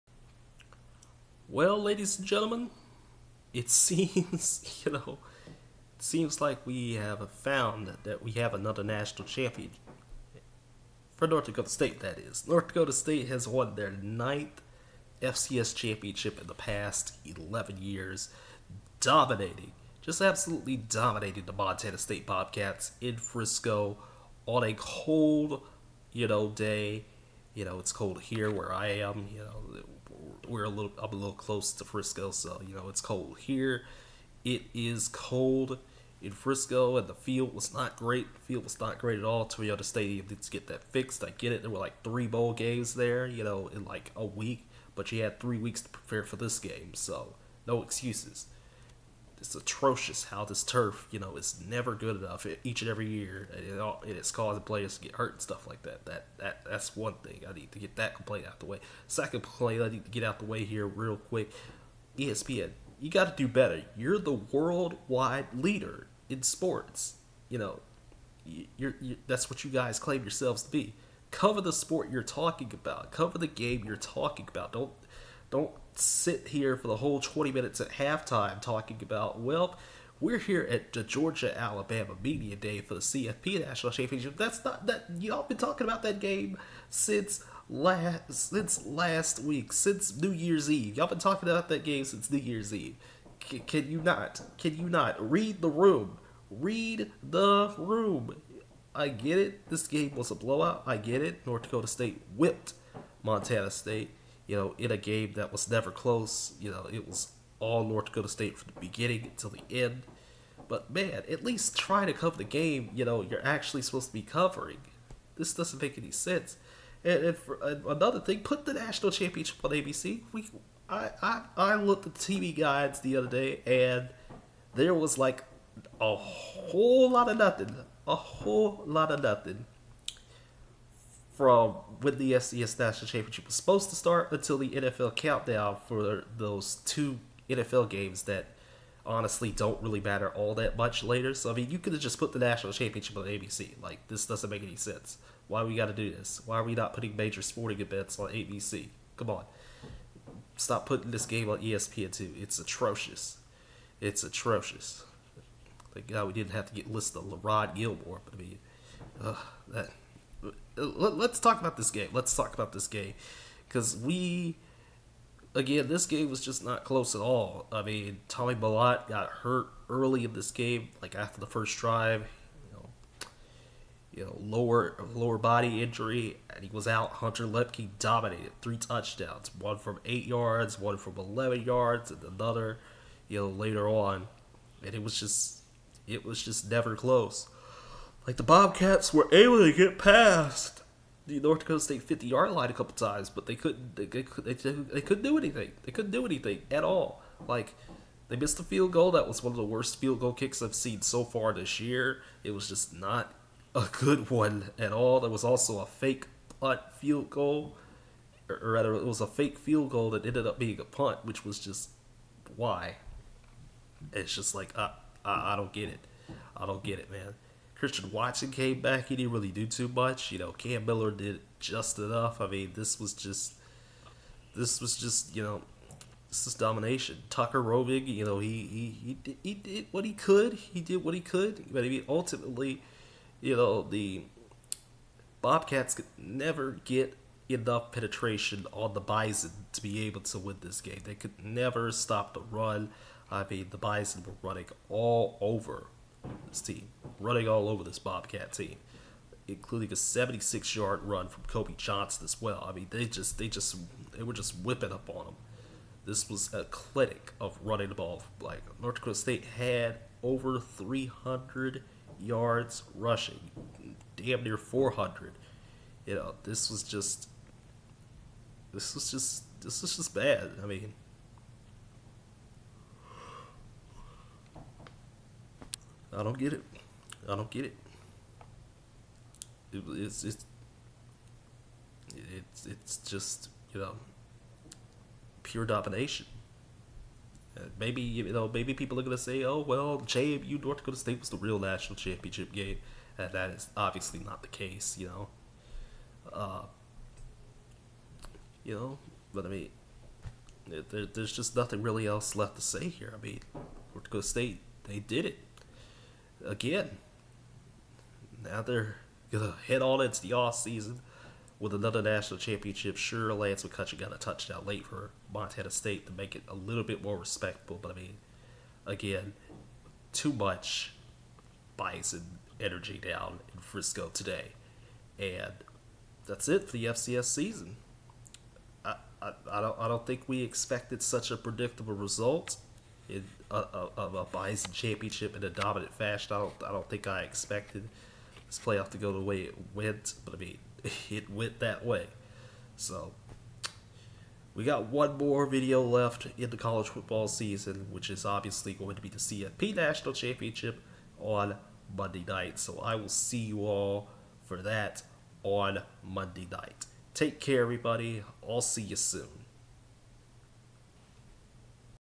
2022 FCS Championship Recap/Reactions
NDSU wins their 9th title in 11 years easily